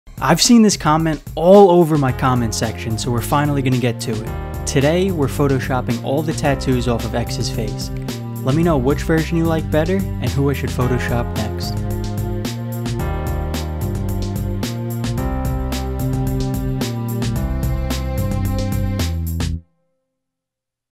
Photoshopping XXXTENTACION's face tattoos off...
XXXTENTACION with no face tattoos. Today we photoshopped all of XXXTENTACION's face tattoos off. Who should I photoshop next?

Jahseh Dwayne Ricardo Onfroy, known professionally as XXXTentacion, was an American rapper, singer, and songwriter.